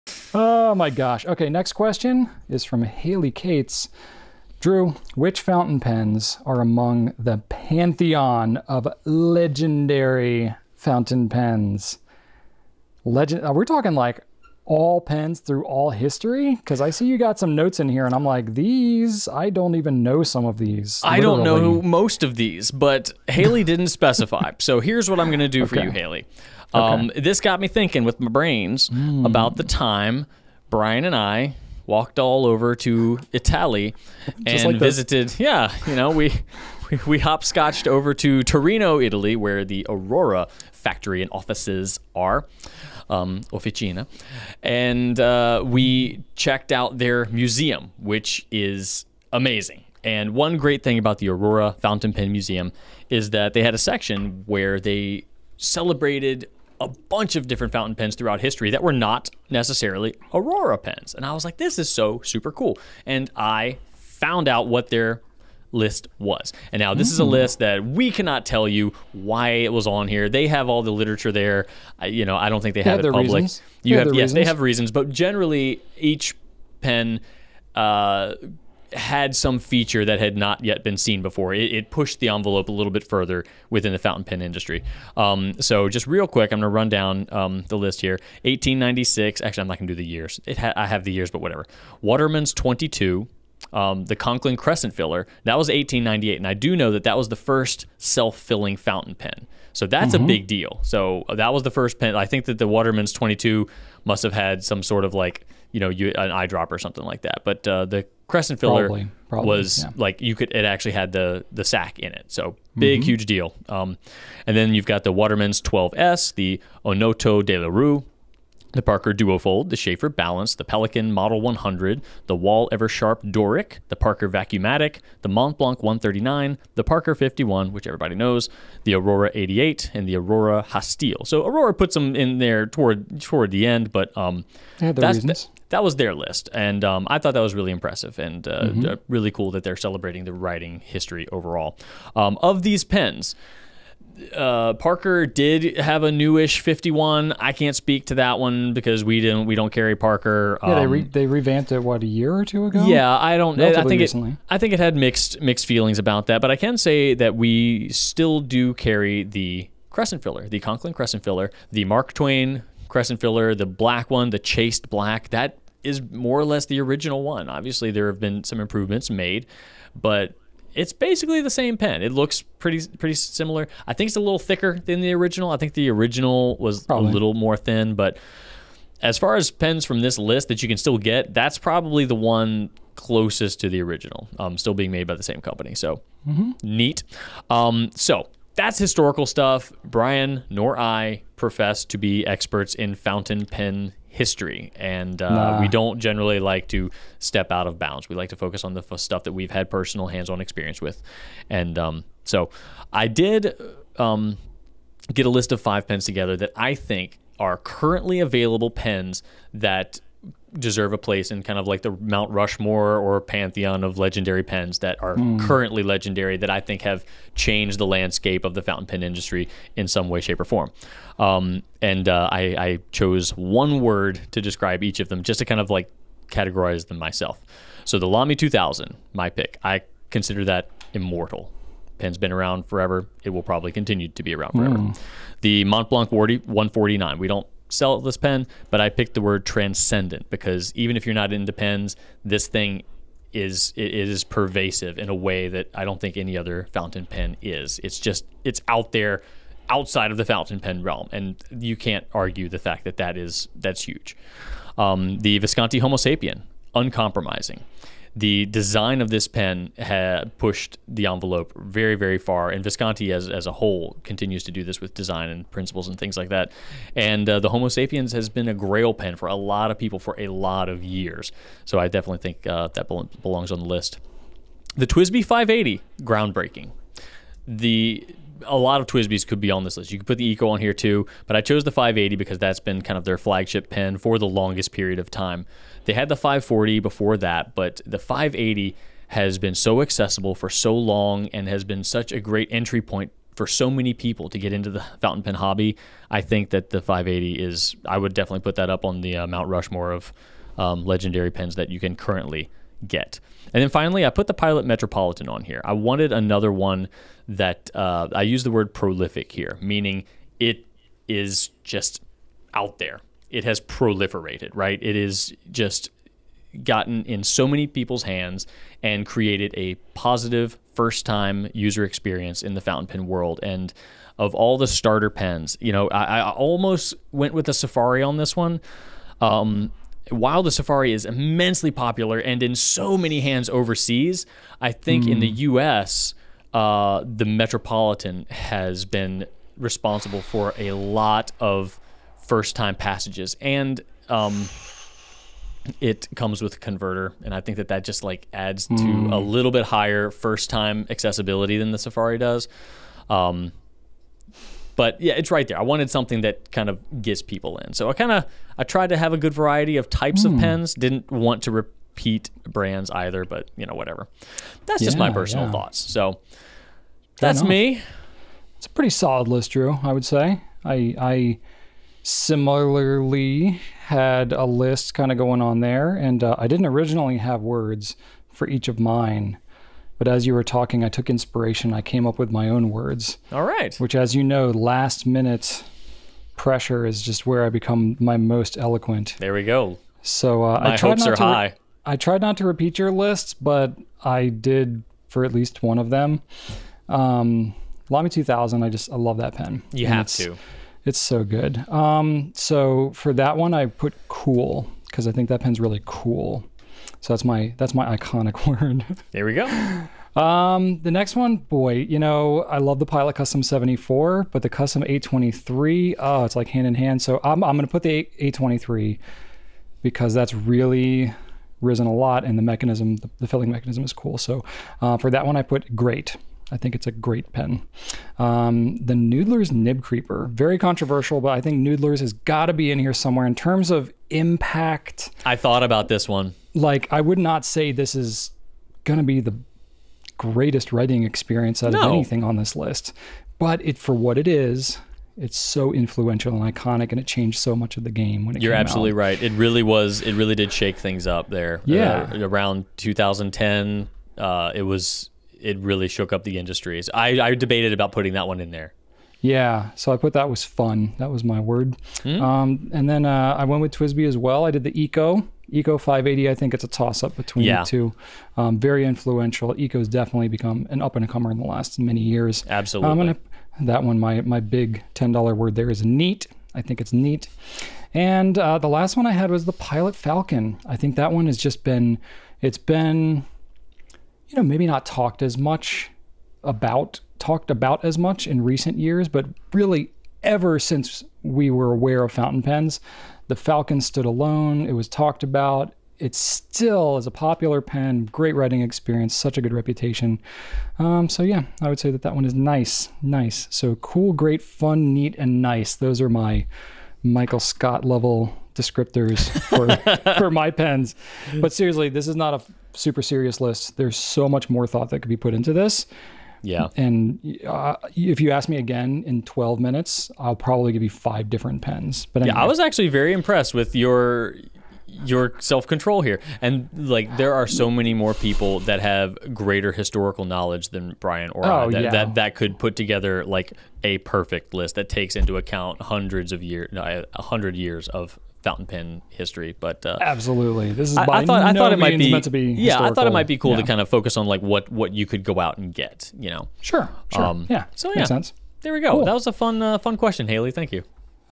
Can We Select a Pantheon of Legendary Pens?
A question for the ages! Which fountain pens are considered to be "legendary"? Brian and Drew do their best to tackle this controversial topic without arguing with themselves too much. Originally from The Goulet Pencast episode 32

TIMESTAMPS:
0:00 The question
0:26 Historical considerations
3:22 Modern considerations from Drew
6:08 Modern considerations from Brian